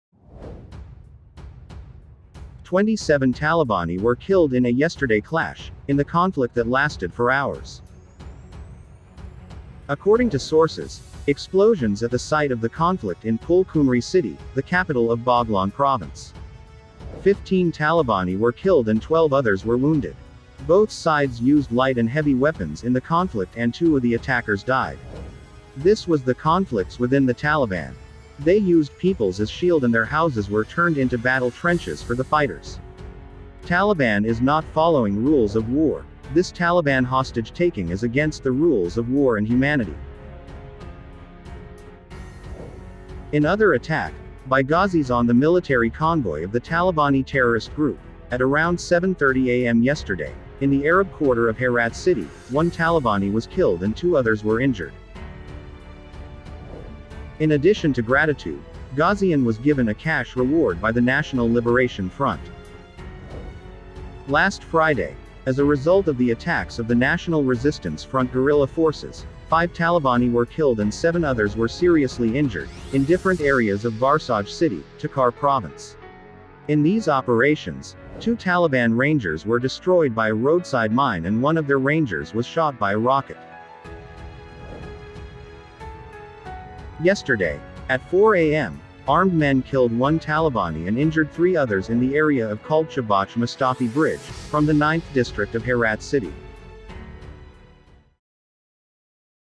Panjshir Updates - Deadly Attack Of NRF Gurrillas, 45 killed
45 talibani killed in total.

27 Talibani were killed in a yesterday clash, in the conflict that lasted for hours,
according to sources explosions at the site of the conflict in Pul Khumri city, the capital of Baghlan province.
15 Talibani were killed and 12 others were wounded.Both sides used light and heavy weapons in the conflict and 2 of the attackers died.
This was the conflicts within the Taliban. they used peoples as shield and their houses were turned into battle trenches for the fighters.
Taliban is not following rules of war, This Taliban hostage-taking is against the rules of war and humanity.
In other attack, by Ghazis on the military convoy of the Talibani terrorist group, at around 7:30 am yesterday, in the Arab quarter of Herat city, one Talibani was
killed and two others were injured.